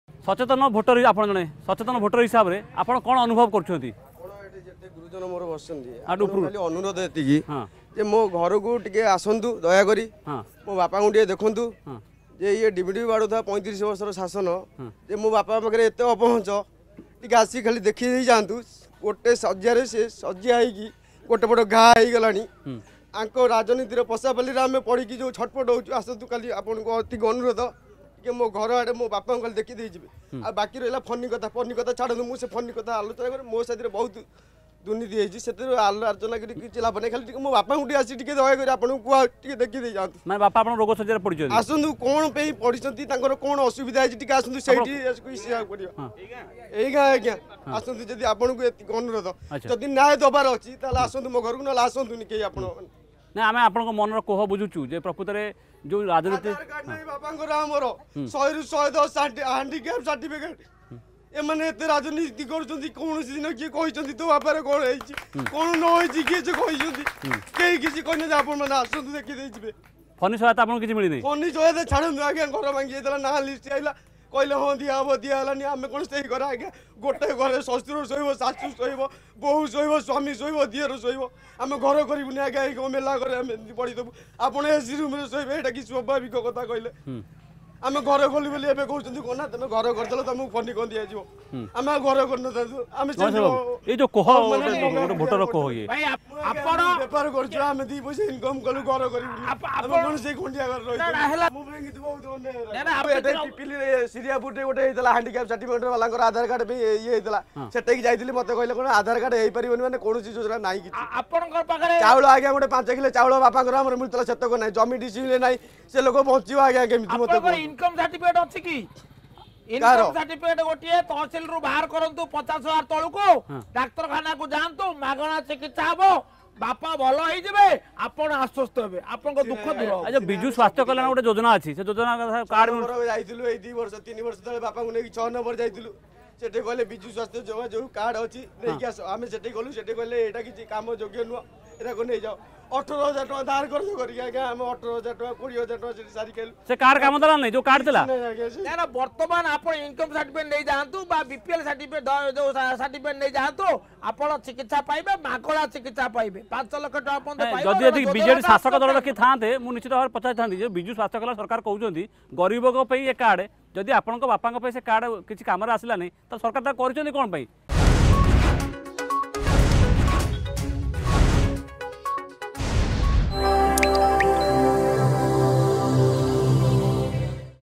କୋହଭରା କଣ୍ଠରେ ଯୁବକଙ୍କ ପ୍ରତିକ୍ରିୟା....
ମୋ ଘରକୁ ଆସନ୍ତୁ ମୋ ବାପାଙ୍କୁ ଟିକେ ଦେଖନ୍ତୁ. ସରକାରୀ ଯୋଜନାରୁ ବଂଚିତ ହେଉଛନ୍ତି ପିପିଲି ଡେଲାଙ୍ଗବାସୀ ଶୁଣନ୍ତୁ କୋହଭରା କଣ୍ଠରେ କଣ କହୁଛନ୍ତି ଯୁବକ.